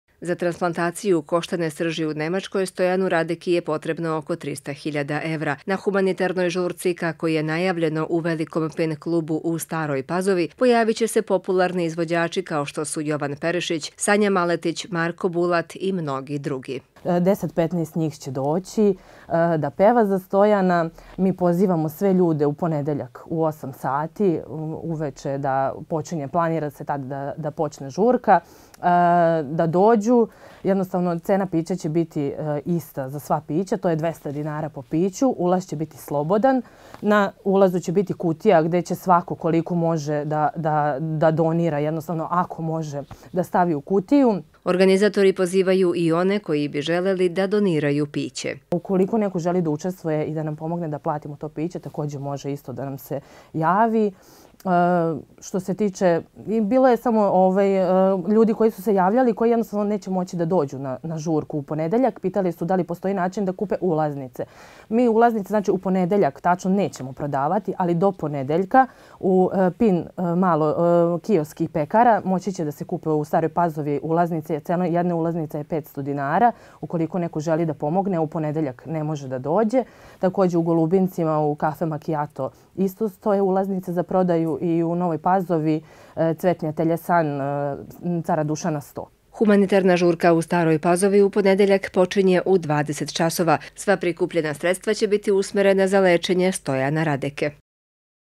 Najava humanitarne žurke
Da nesreće spajaju ljude, pokazuju i najnovije akcije koje se organizuju ovih dana u cilju prikupljanja sredstava za operaciju petogodišnjeg Stojana Radeke iz Stare Pazove.  Za ponedeljak, 6. Februar najavljena je humanitarna žurka u Pin klubu u Staroj Pazovi a koju organizuju prijatelji Stojanovih roditelja.